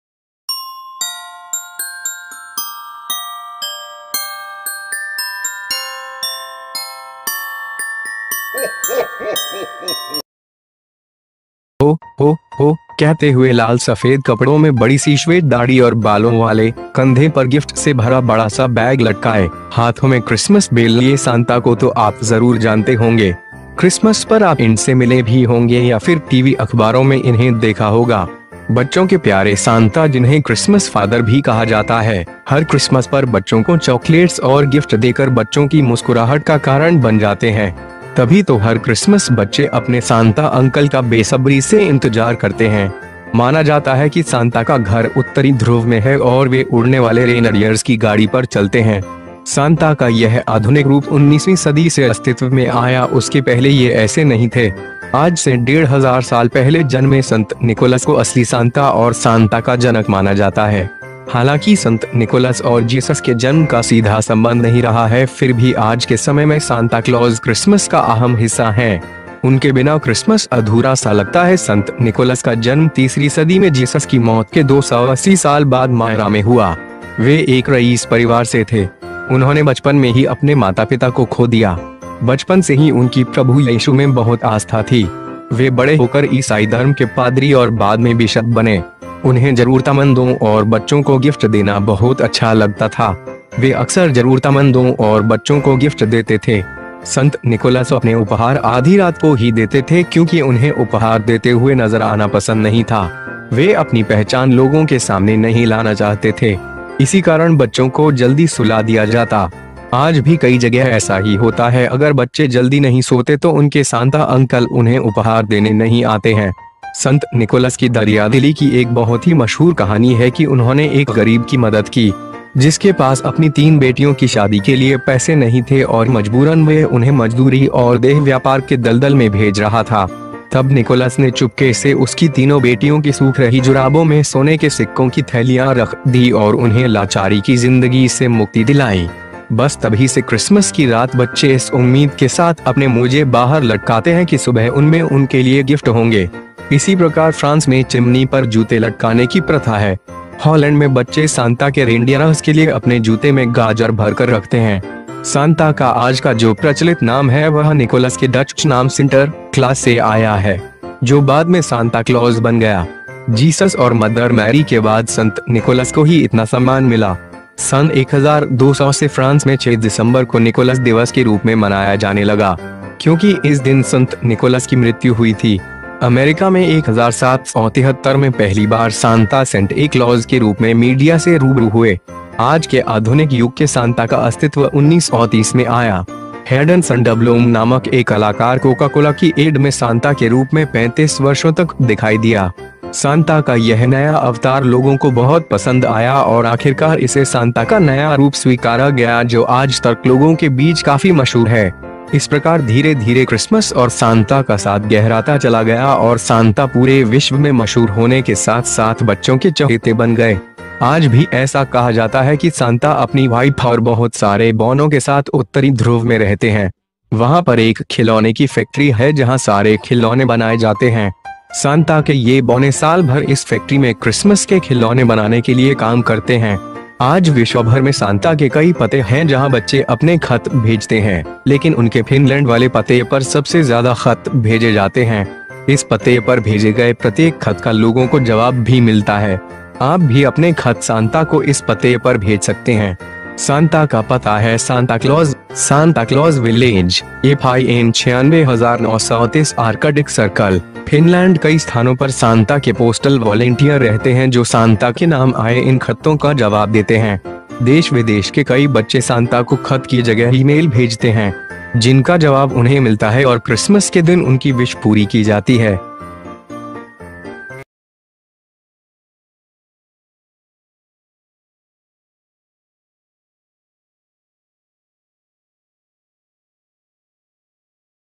Santa Claus कौन थे ? | Interesting Facts and History about Santa Claus in Hindi| Punjabi Christians
Buy Christians T-Shirts, Mobile Covers and Much More...

Best Collection of Christian T-shirts, Mobile Covers & Much More